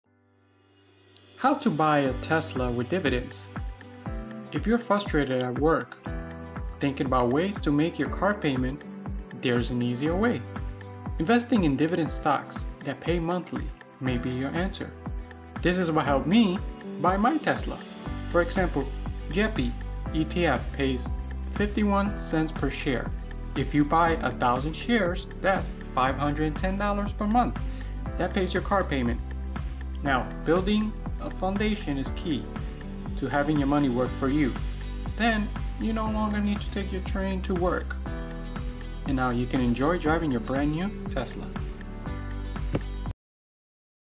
Unbelievable Way to Buy a Tesla WITHOUT Spending a Dime!
In this video, we'll show you how to get a Tesla without spending a dime!

Tesla cars are some of the most popular and luxurious vehicles on the market, but they can be a bit expensive to buy. In this video, we're going to show you a way to buy a Tesla without spending a single penny! By using a sneaky technique called "backdoor buying", you can get a Tesla without spending a single penny!

Booked that helped me!